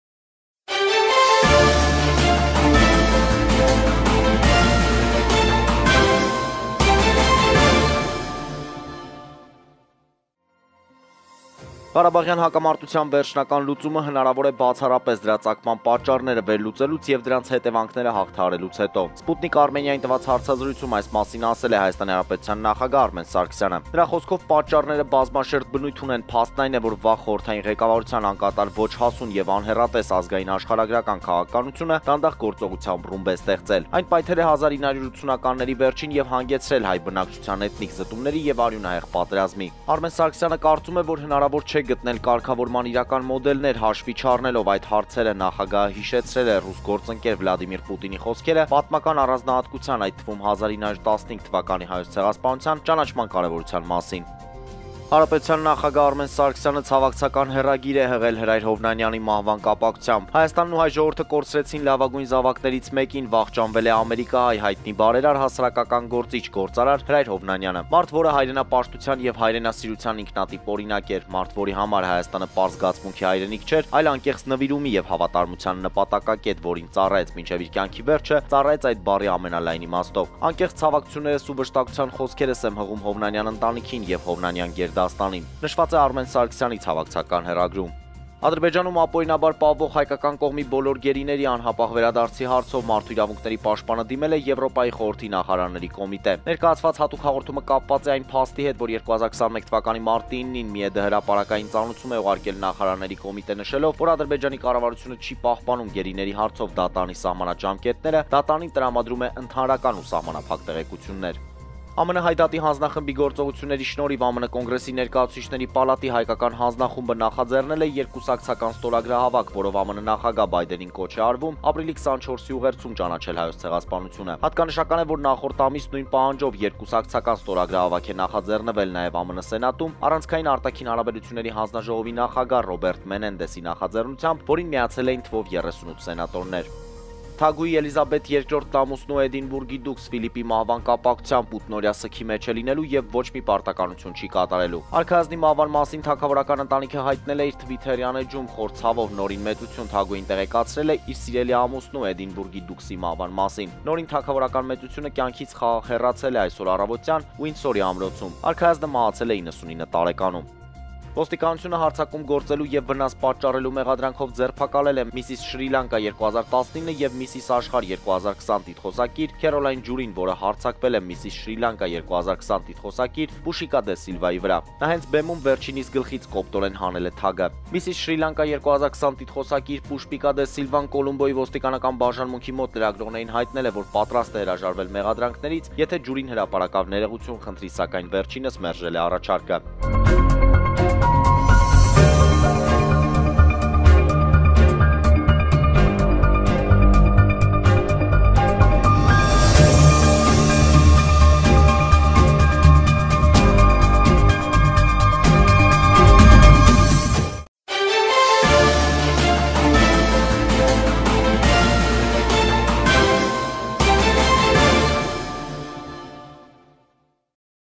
Հայ գերիների անհապաղ վերադարձի հարցով ՄԻՊ-ը դիմել է Եվրոպայի խորհրդի նախարարների կոմիտե
Բաժանորդագրվեք մեր նոր ալիքին։